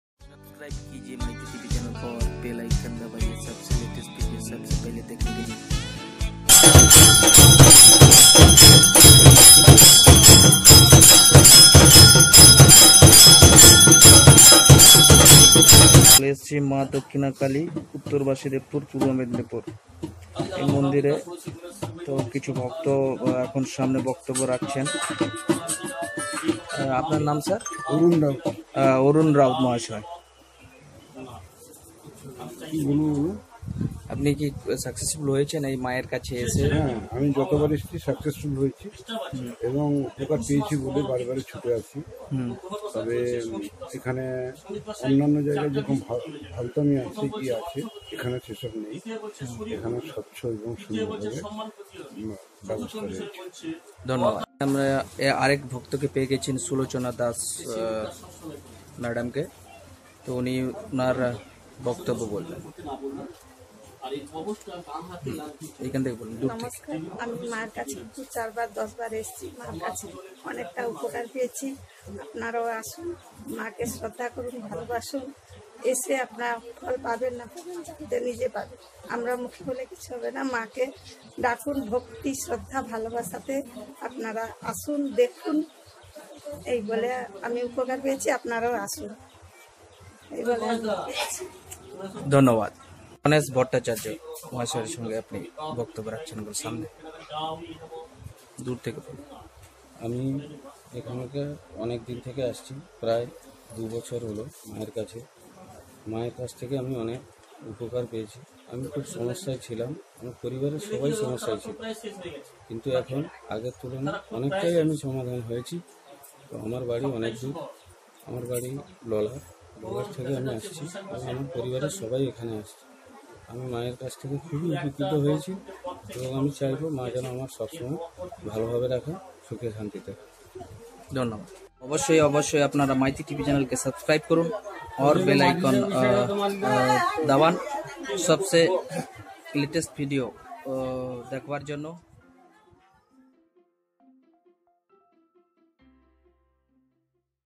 History of#Dhakshina_KaliMata#North_Basudebpur#BhavaniChack Interview Part-2#Live_Stream
Subject:History of#Dhakshina_KaliMata#North_Basudebpur#BhavaniChack Interview Part-2#Live_Stream
Description:  Interview.